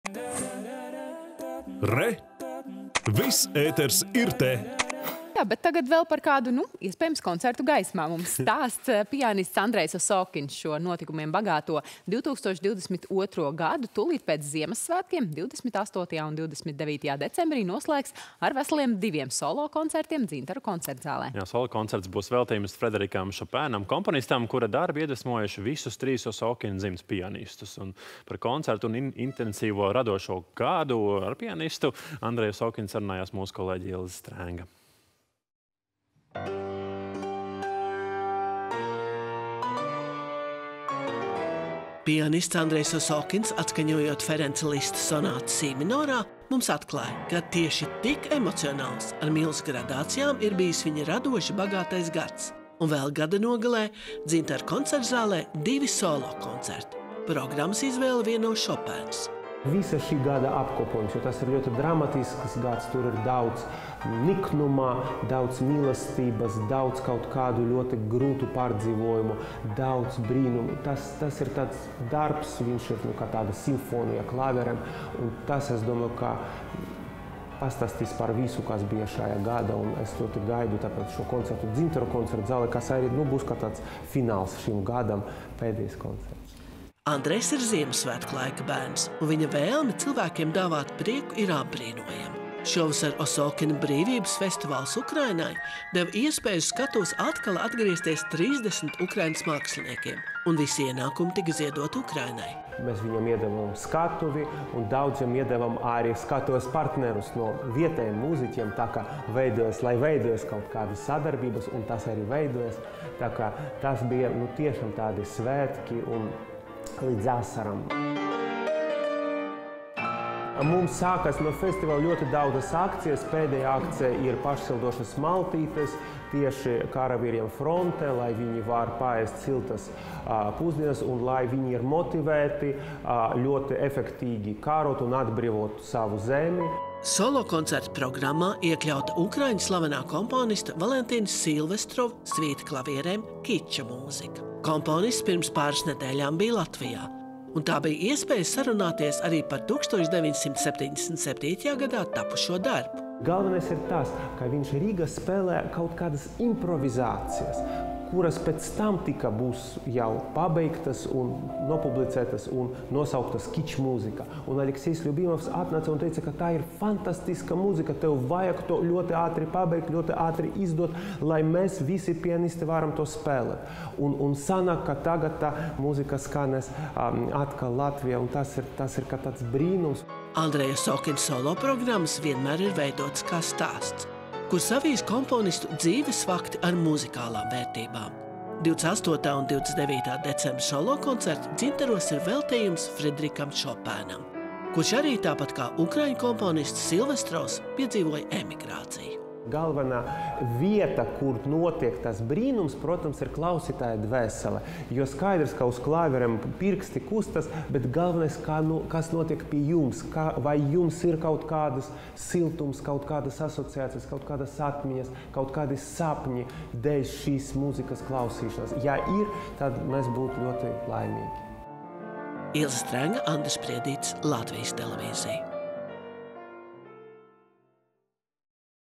Veltījums Šopēnam. Pianista Andreja Osokina solokoncerti
Pianists Andrejs Osokins šo, notikumiem bagāto  2022.gadu   tūlīt pēc Ziemassvētkiem 28.un 29.decembrī, noslēgs ar diviem solokoncertiem Dzintaru koncertzālē.
  Solokoncerts būs  veltījums Friderikam Šopēnam, komponistam, kura darbi iedvesmojuši visus trīs Osokinu dzimtas pianistus.
   Par solokoncertu un intensīvo radošo gadu ar pianistu Andreju Osokinu sarunājās žurnāliste Ilze Strenga.